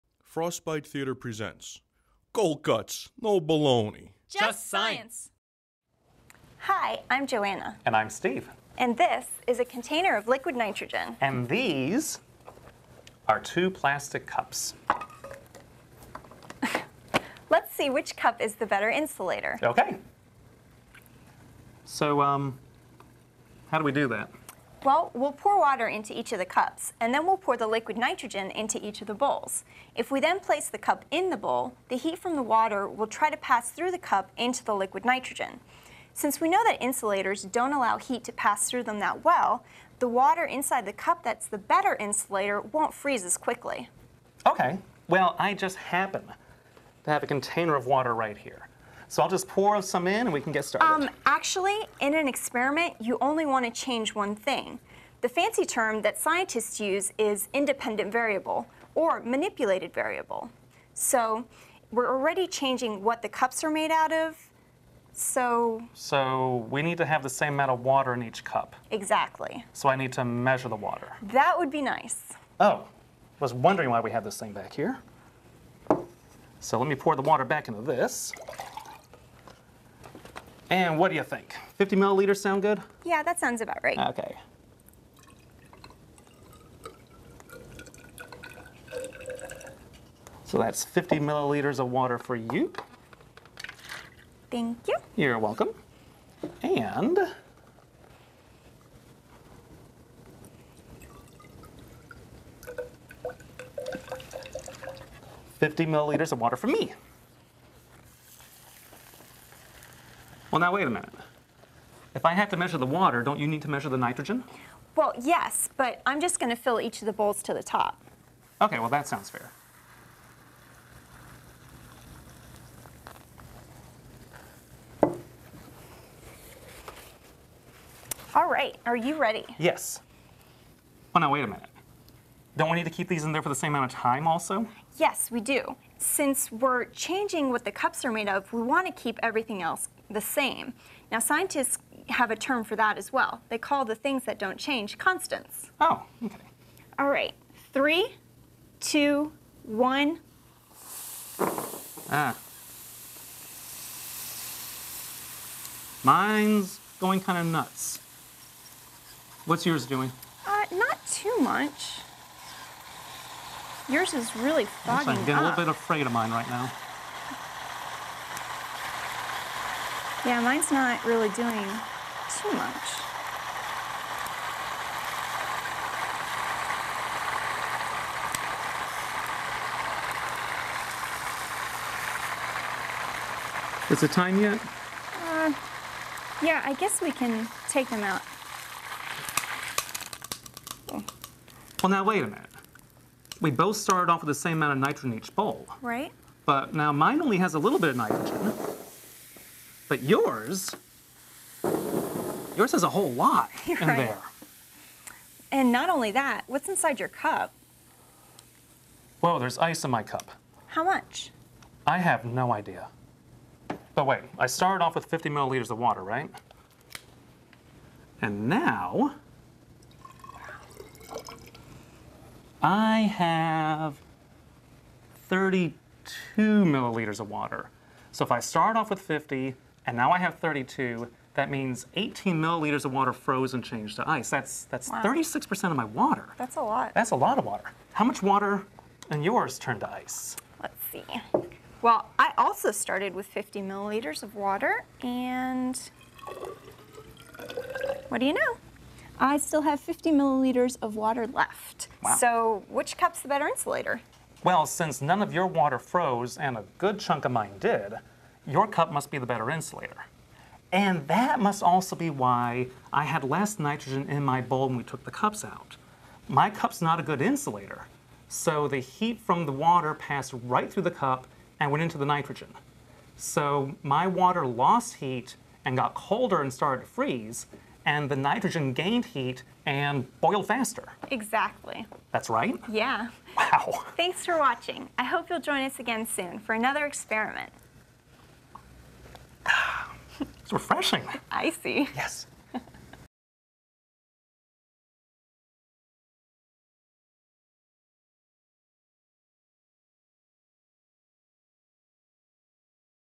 Liquid Nitrogen Experiments: Insulators
Cups full of water are placed into bowls of liquid nitrogen! Which cup will insulate the best? [Closed Captioned]